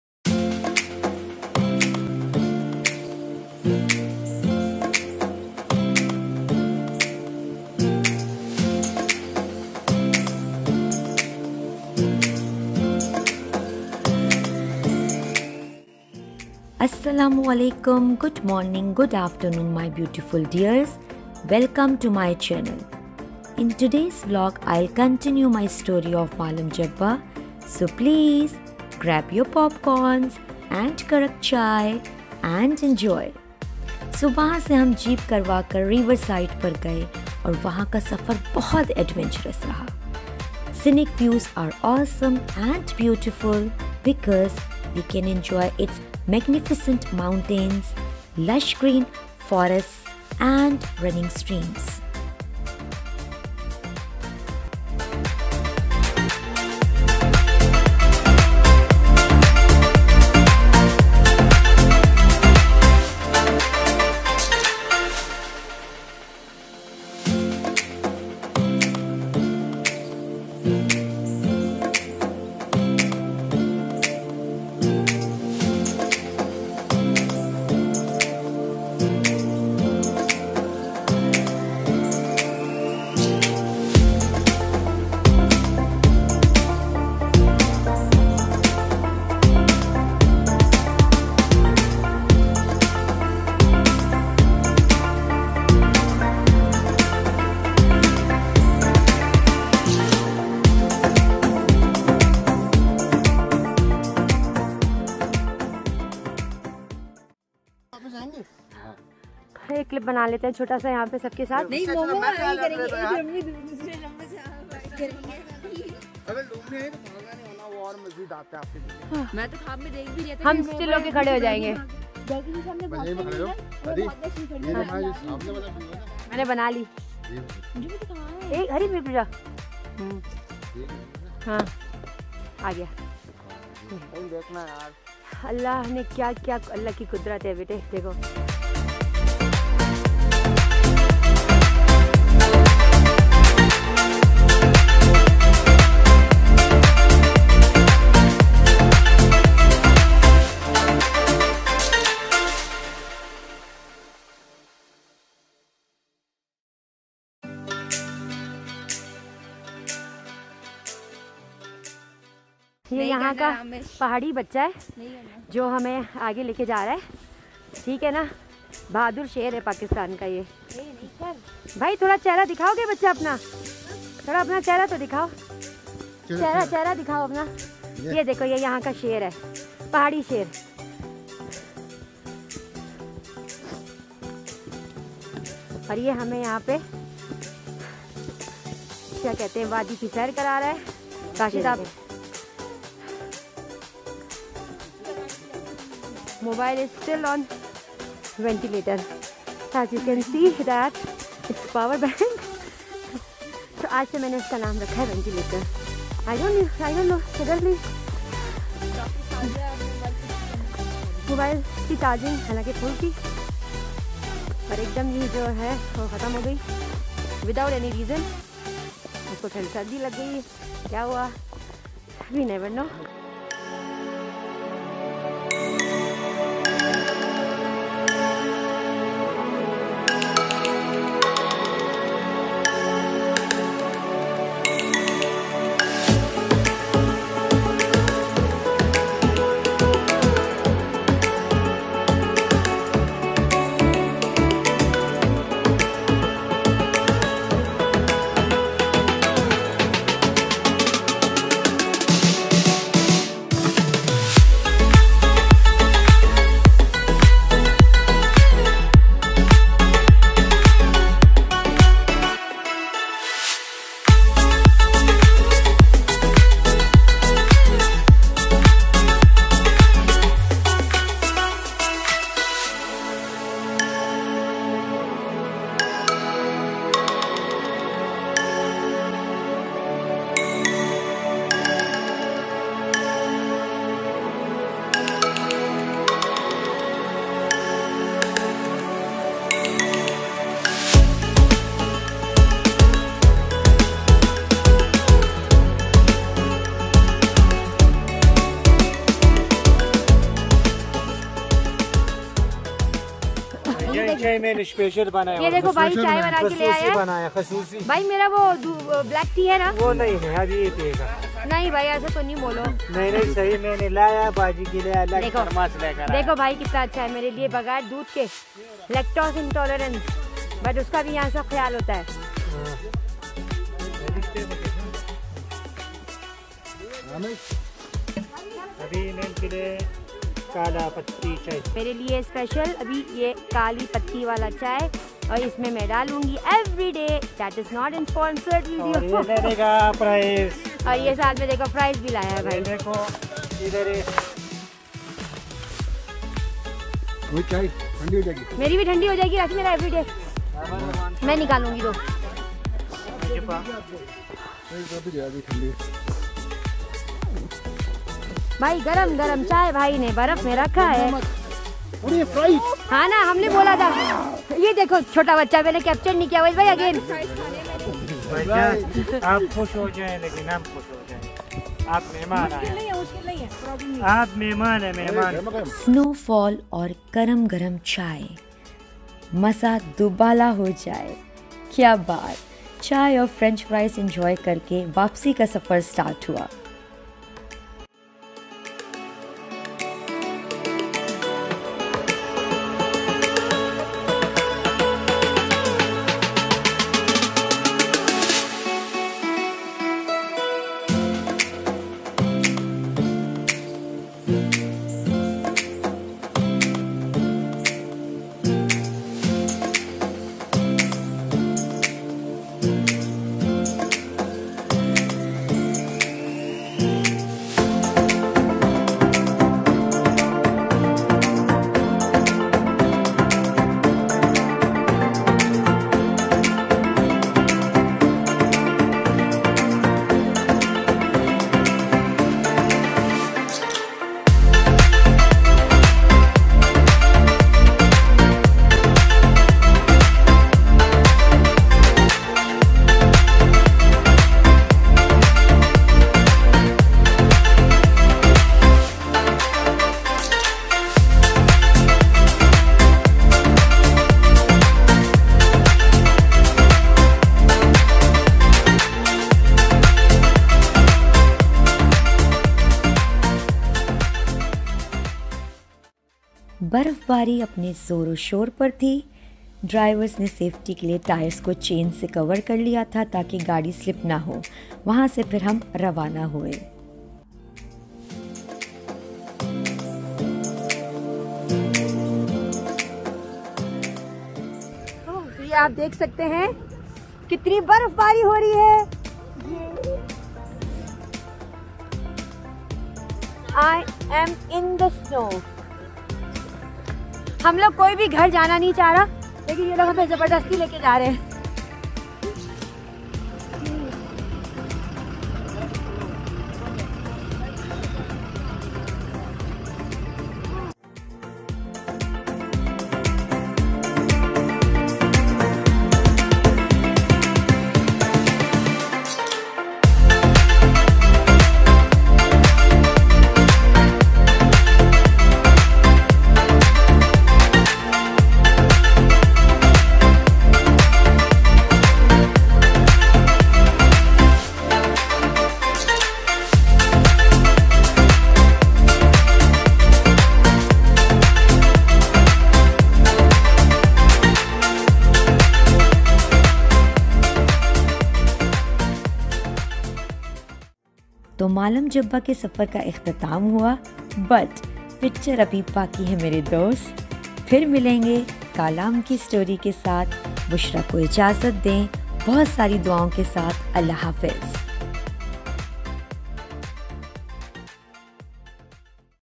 Northern Areas Of Pakistan-Travel Vlog 4 - Malam Jabba Snowfall Part -2 2022
Hello dearssss!
Recently I visited Northern Areas of Pakistan from DUBAI.I want to share my experience for overseas Pakistani. Because there are so many facts and information which I want to share with you, So you can enjoy your trip.
Vlog-5 is coming soon.